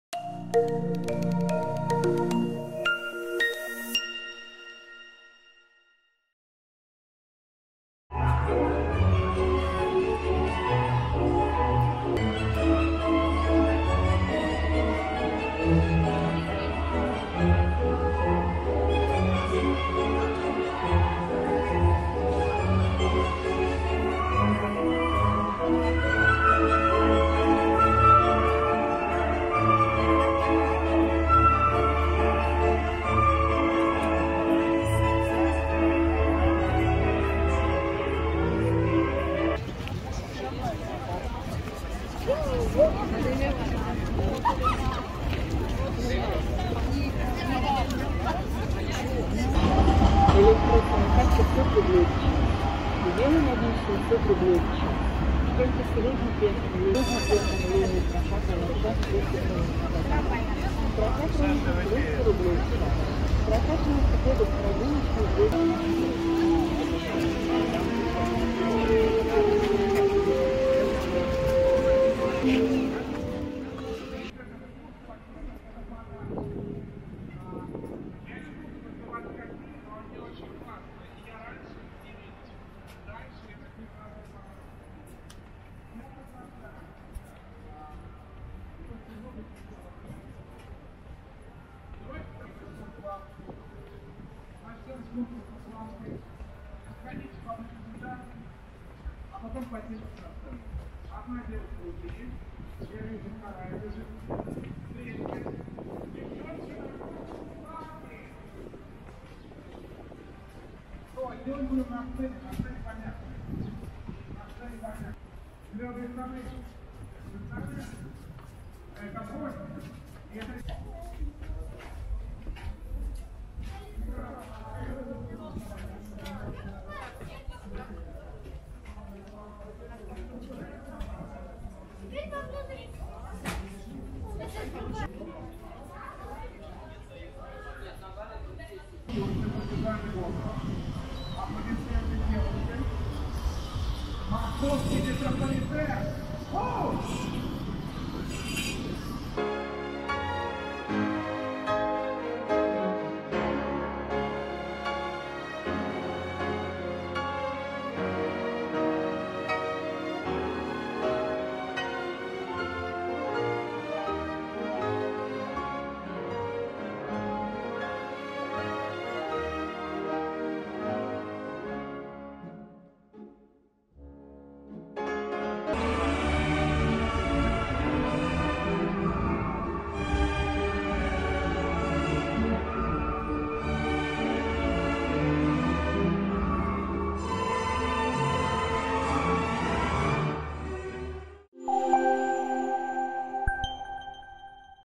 [서울디자인리포터] ep 4. 베덴하 재건 프로젝트 – 뫼비우스띠 | 도시재생 디자인
베덴하 뫼비우스띠는 2023년 6월에 개장한 비정형 건축 구조의 산책로입니다.
서울의 DDP와 유사한 모습을 갖춘 이 건축물이 어떤 모습을 하고 있는지 러시아 모스크바에서 문용원 리포터가 소식을 전달합니다.